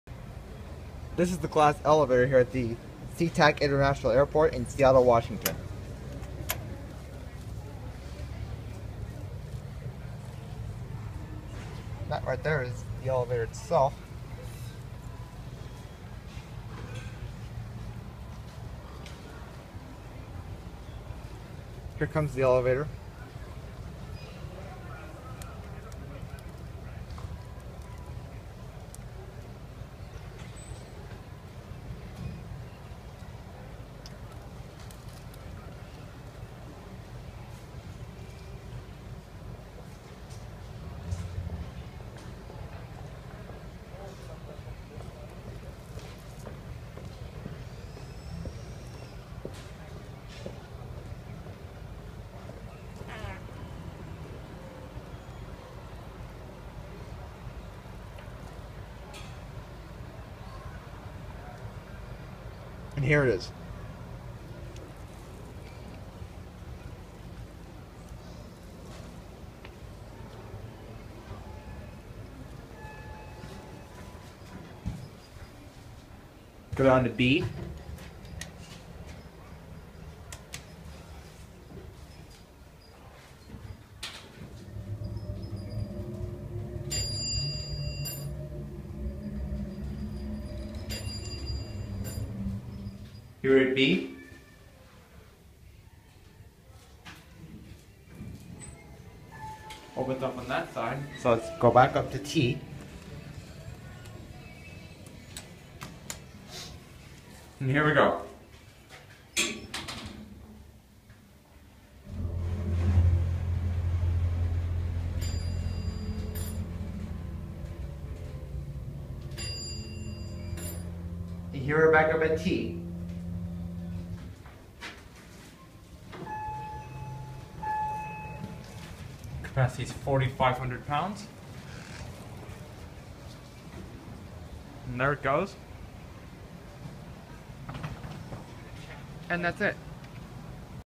(Unknown Brand) Hydraulic Glass Elevator At Seattle-Tacoma International Airport - SeaTac, WA
Brand: Unknown
Type: Hydraulic
Capacity: 4500 lbs.
Fixtures: Unknown
Motor: Unknown
Total Floors: 3 (B, P, T)